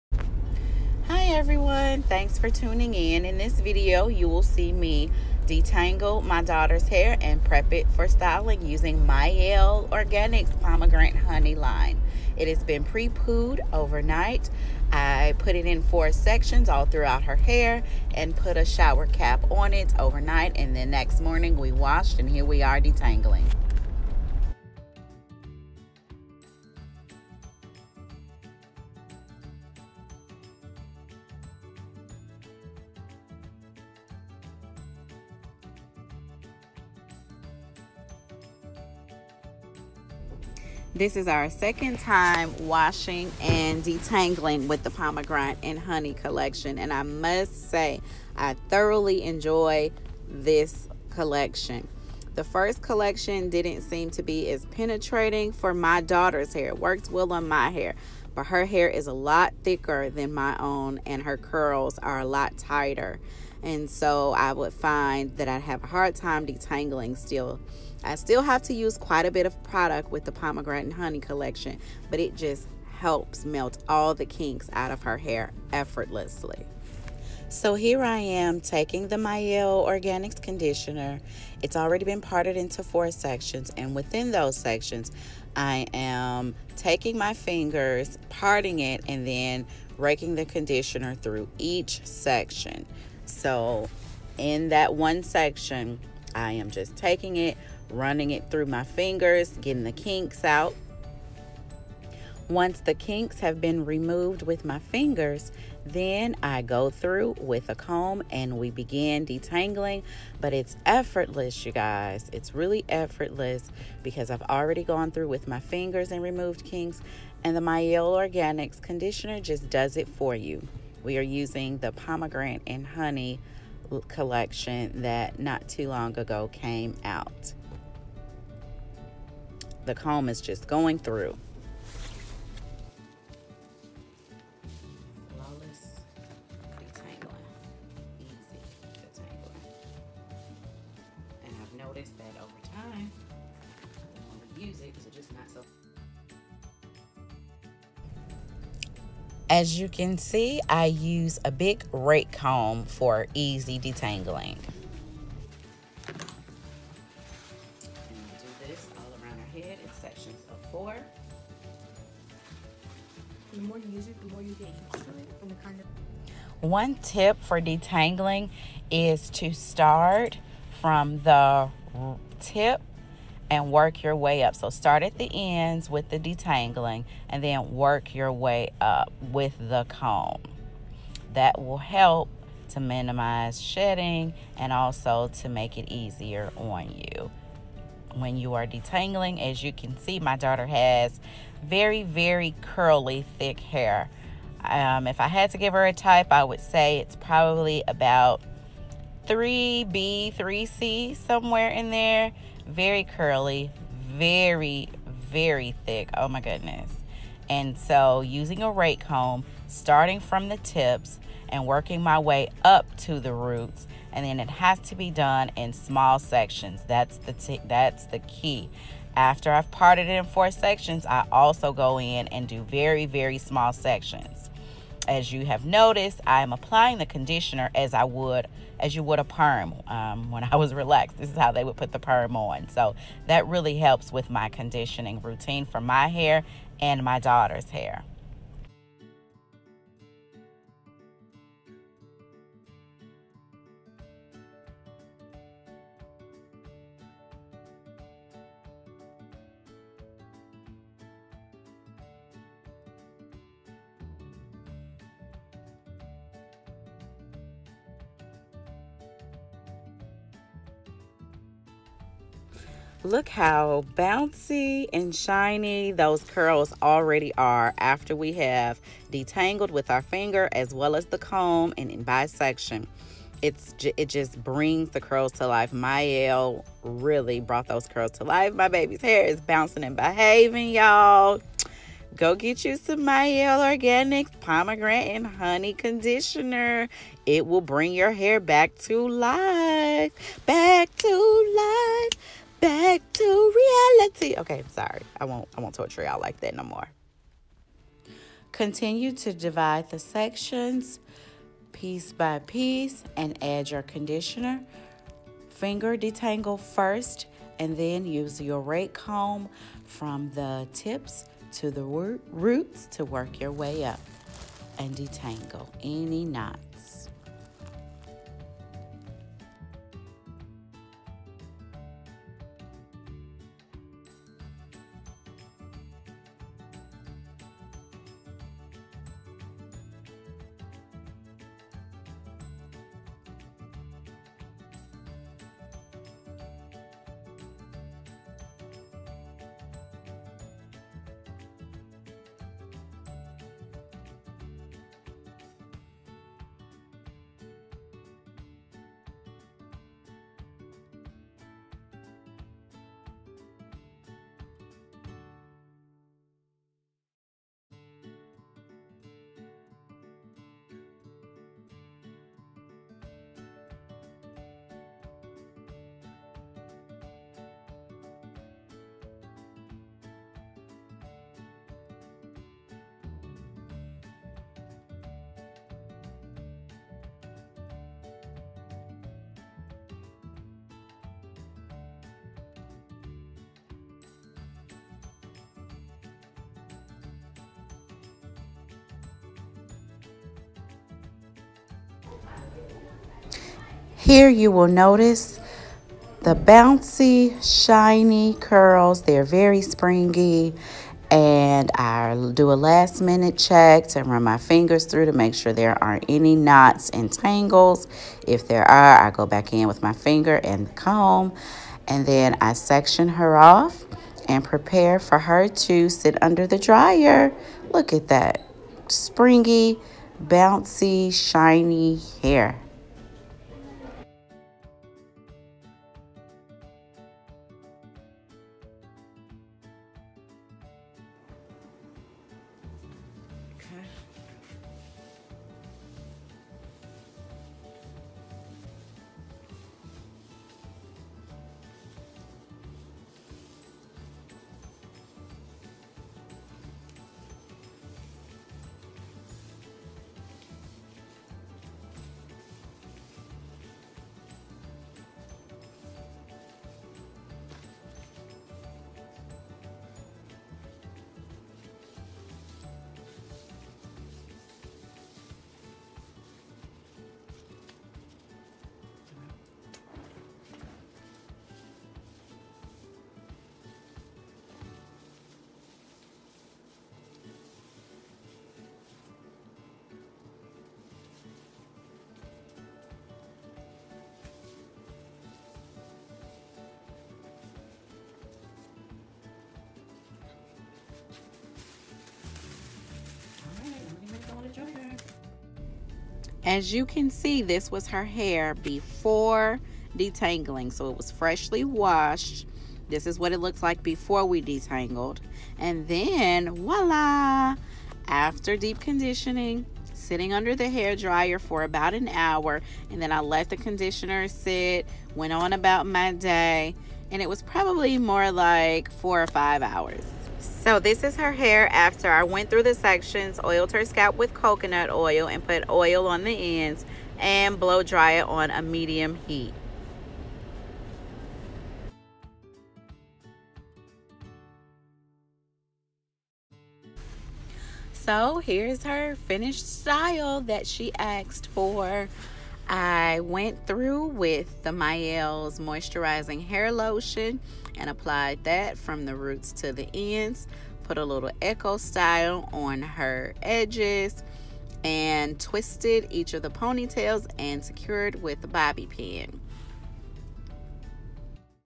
Fast, Easy Detangling Routine Ft Mielle Organics Pomegranate and Honey Collection
This is my detangling routine for my daughter's hair. Watch how I detangle, deep condition and prep her hair to style. Mielle has taken our detangling routine from 45 minutes, to 20 minutes!!

Let's be friends:

Snap: Only1Adreean

Pomegranate and Honey Moisturizing  and Detangling Conditioner

Pomegranate and Honey Moisturizing  and Detangling Shampoo

White Peony Leave In Conditioner

Avocado Hair Milk

Other Products:
Simply Nature Organic Coconut Oil (purchased at Aldi)
Eco Style gel- Olive Oil line